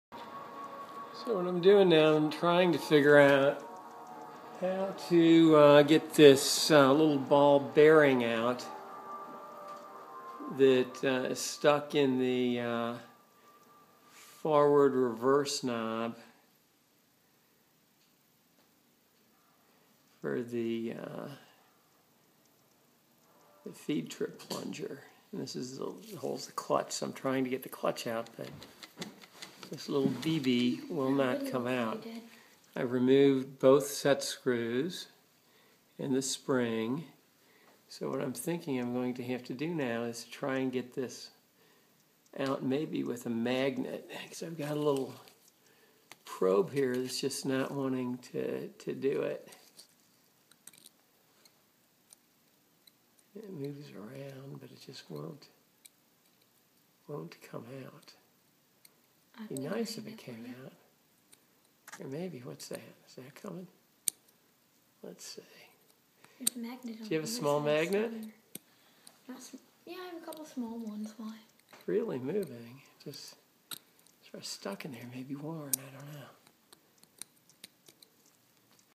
Bridgeport quill head tear down part 4
Bridgeport quill head disassembly.  How I removed the stuck BB ball in clutch.